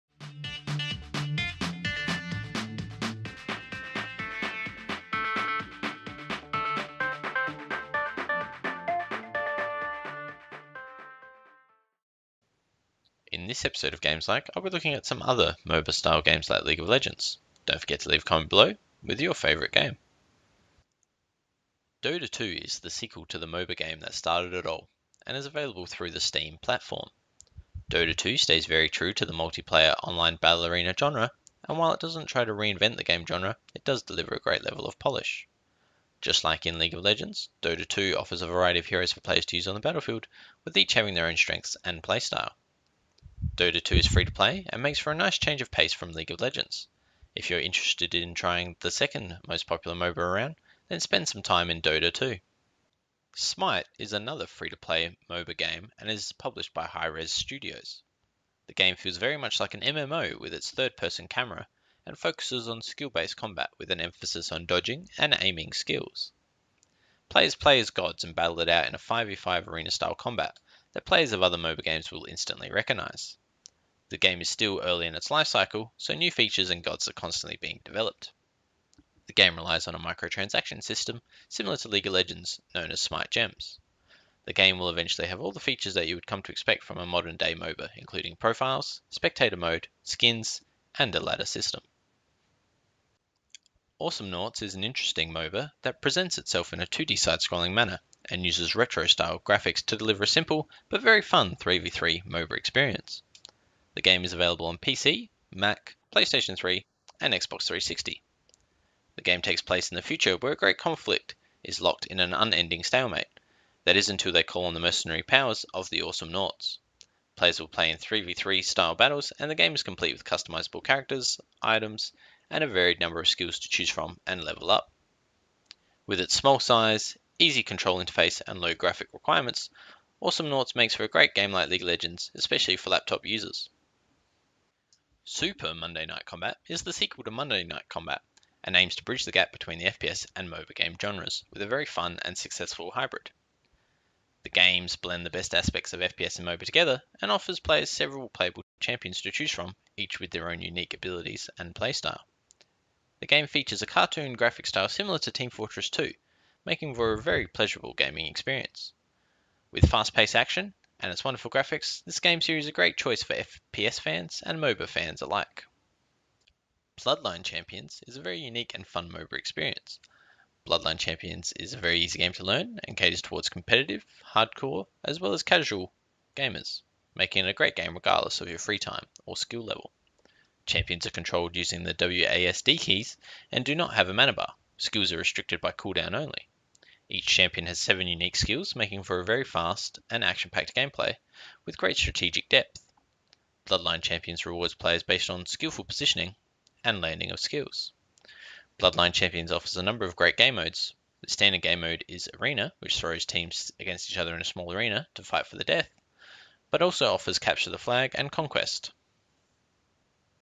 Games Like League of Legends - Best MOBA Games
Looking for the best MOBA games? Check out this collection of games like League of Legends.

These best MOBA games are not featured in any particular order and may not appeal to everyone.